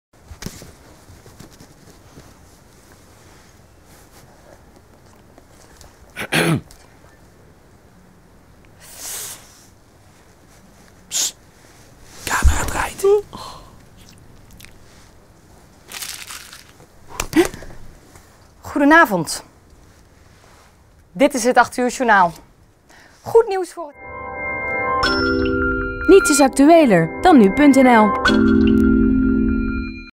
nu.nl 'Niets is actueler dan nu.nl!'
Client: nu.nl (dutch daily news content site).
Brief: An established news anchor gets caught reading latest and most up to date news on nu.nl instead of her own news.

Media: TV, online. Agency: Noise BV Amsterdam.
Artdirection: Dave Fikkert.
Production: Bananaz Amsterdam.